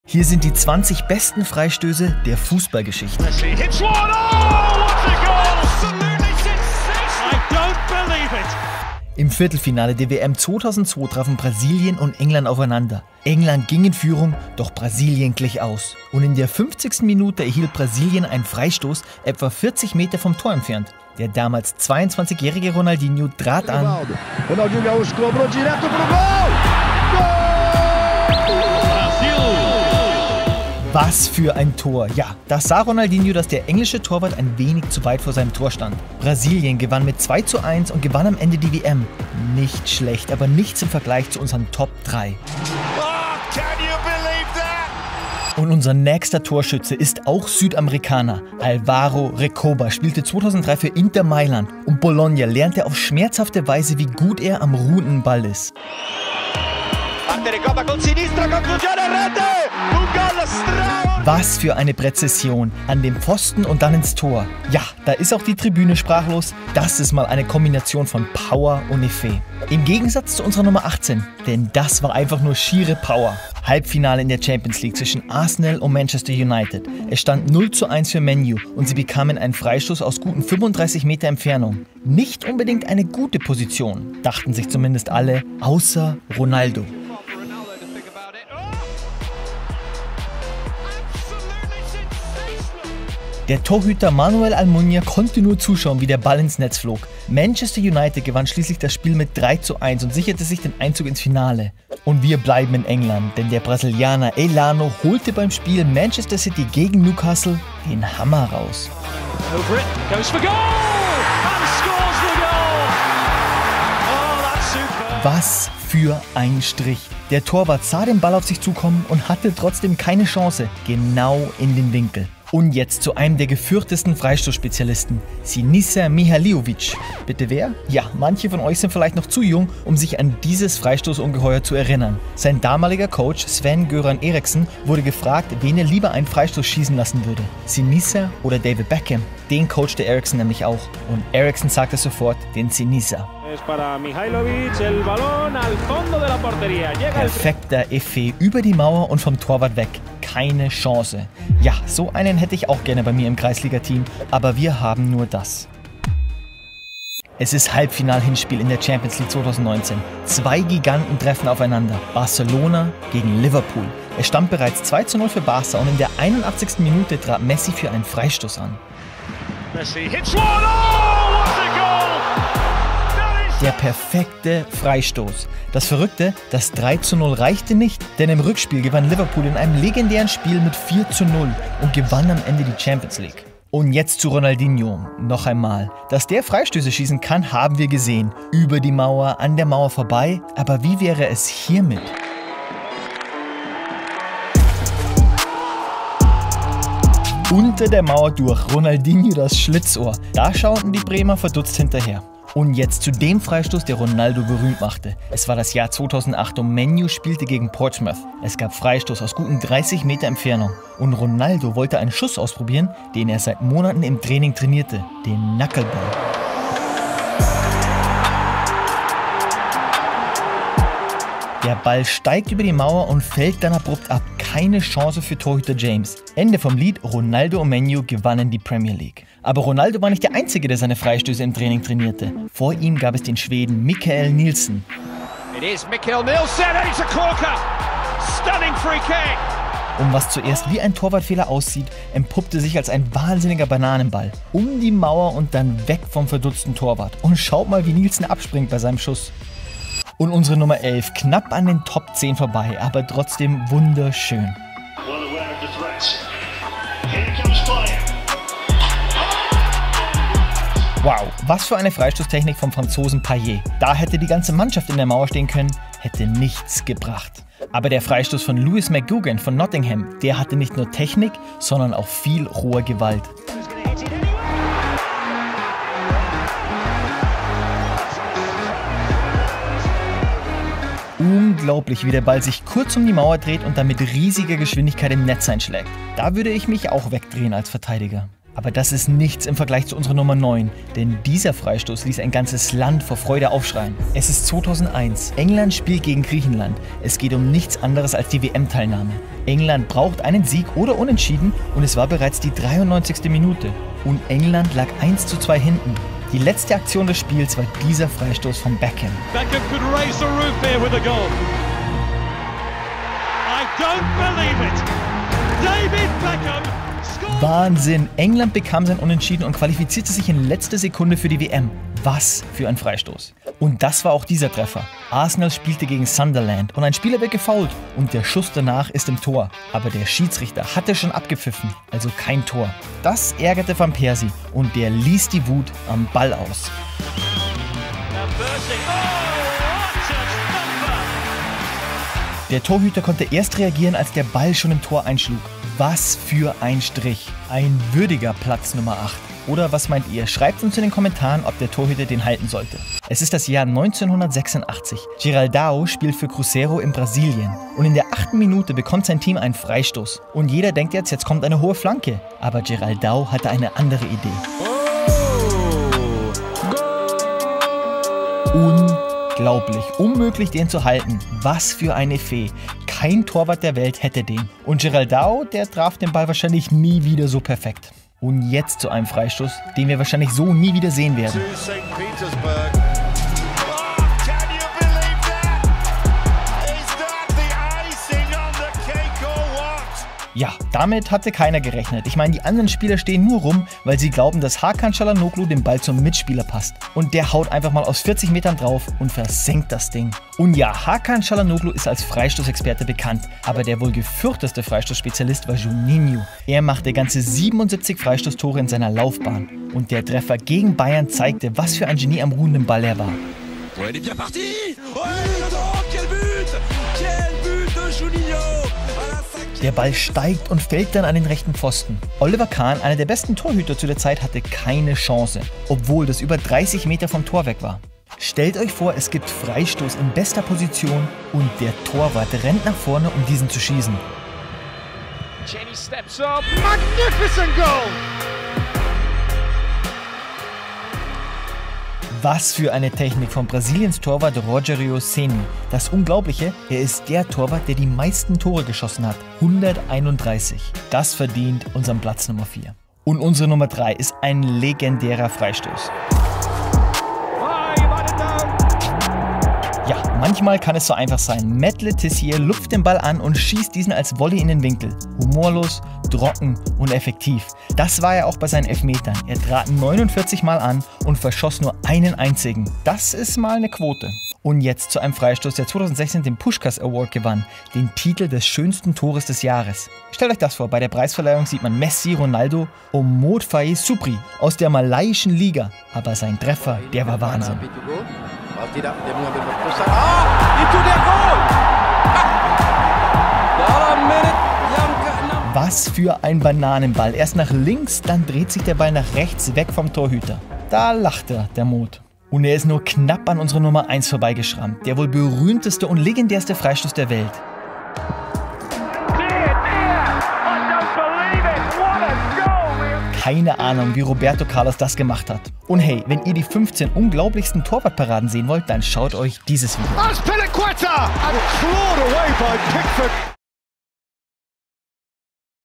Die 20 BESTEN Freistoß-Tore im Fußball
Hier sind 20 unglaubliche Freistöße. Liste ist natürlich subjektiv:)
Welche ist euer Favorit?